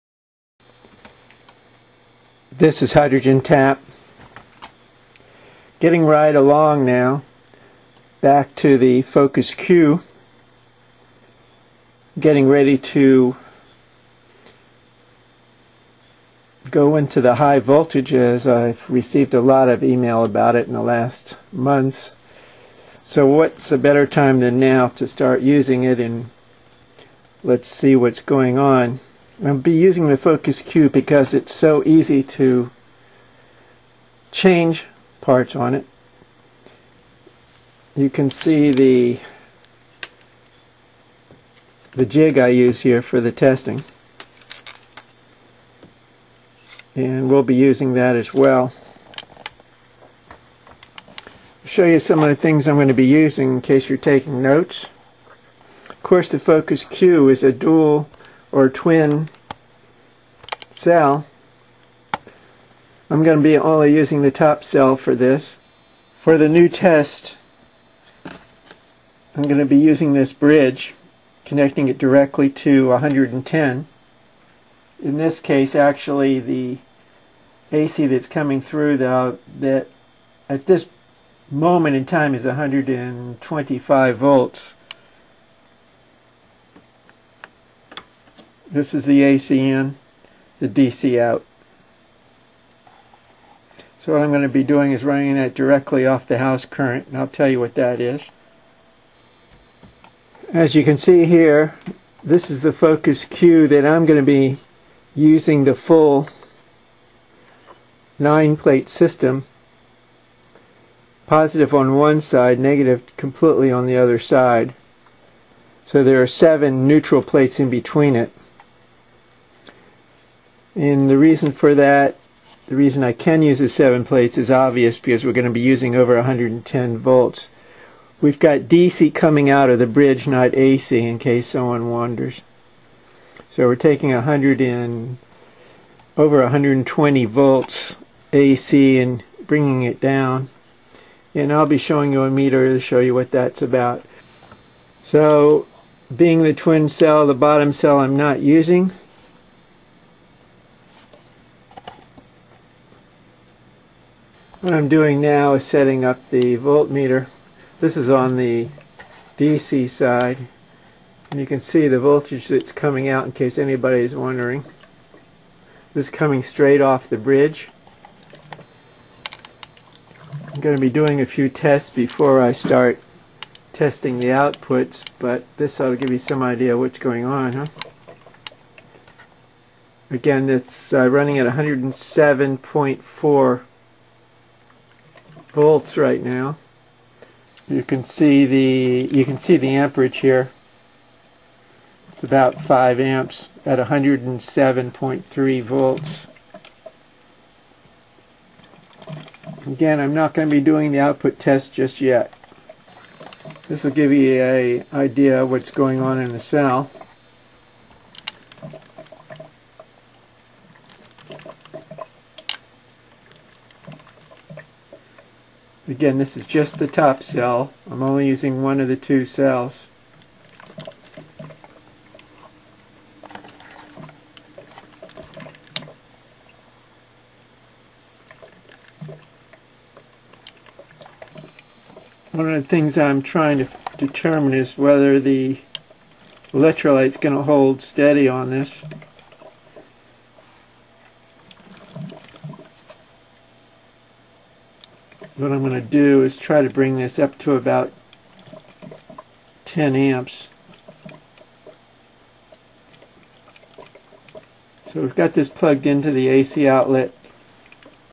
Hydrogen Tap (FOCUS-Q 107 VOLTS) 408
This video shows a new test set up. The "Q" will be run on 107 volts (DC) using a Bridge.